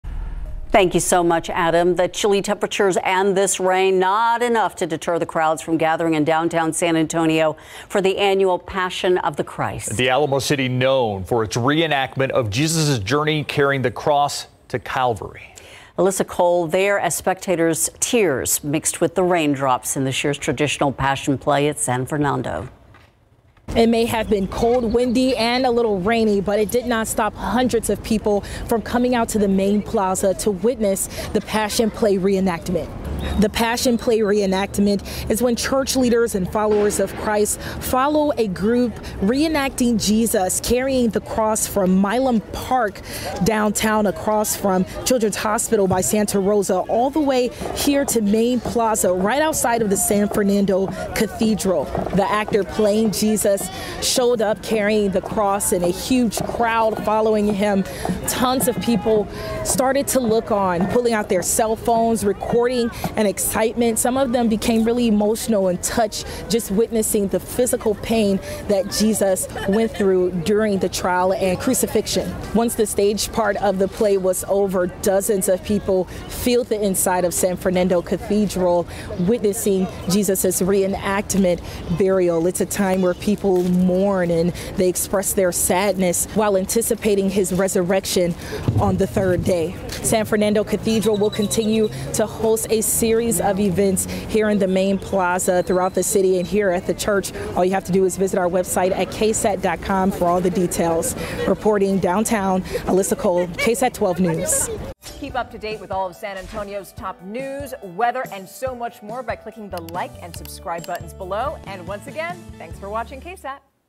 San Antonians line downtown streets to watch Passion Play
People lined the streets of downtown San Antonio for the Good Friday tradition that recreates the trial and crucifixion of Jesus Christ — the Passion Play.